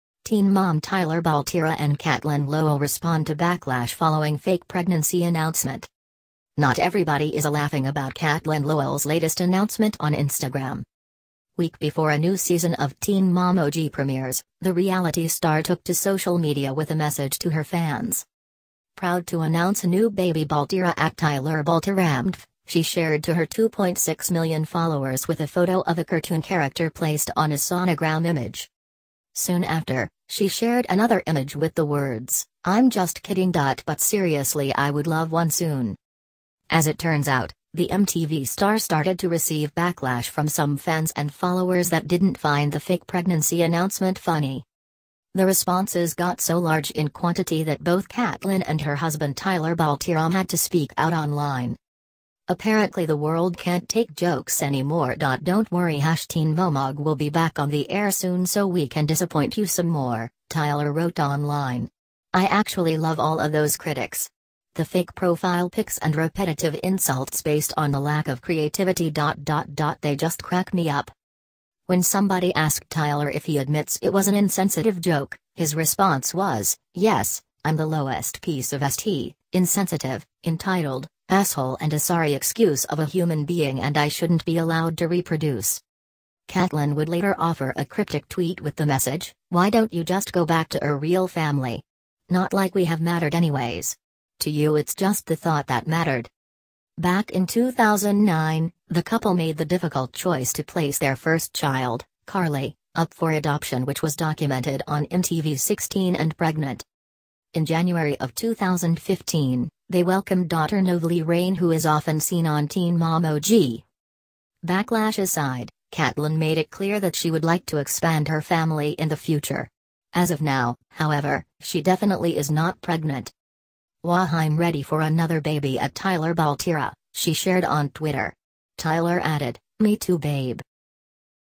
Teen MomTyler Baltierra and CatelynnLowell Respond to Backlash Following Fake Pregnancy Announcement
Not everybody is laughing about Catelynn Lowell's latest announcement on Instagram.

Week before a new season of Teen Mom OG premieres, the reality star took to social media with a message to her fans.

Proud to announce a new baby Baltierra @tylerbaltierramtv, she shared to her 2.6 million followers with a photo of a cartoon character placed on a sonogram image.

Soon after, she shared another image with the words, I'm just kidding…but seriously I would love one soon.

As it turns out, the MTV star started to receive backlash from some fans and followers that didn't find the fake pregnancy announcement funny.
The responses got so large in quantity that both Catelynn and her husband Tyler Baltierra had to speak out online.

Apparently the world can't take jokes anymore…don't worry TeenMomOG will be back on the air soon so we can disappoint you some more, Tyler wrote online. I actually love all of those critics. The fake profile pics & repetitive insults based on the lack of creativity...they just crack me up.

When somebody asked Tyler if he admits it was an insensitive joke, his response was, Yes, I'm the lowest piece of s--t, insensitive, entitled, asshole & a sorry excuse of a human being & I shouldn't be allowed to reproduce.
Catelynn would later offer a cryptic tweet with the message Why don't u just go back to ur real family Not like we have mattered anyways.. to u it's just the thought that mattered.

Back in 2009, the couple made the difficult choice to place their first child, Carly, up for adoption which was documented on MTV's 16 and Pregnant.

In January of 2015, they welcomed daughter Novalee Reign who is often seen on Teen Mom OG.
Backlash aside, Catelynn made it clear that she would like to expand her family in the future. As of now, however, she definitely is not pregnant.

Wahhh I'm ready for another baby @TylerBaltierra, she shared on Twitter. Tyler added, Me too babe.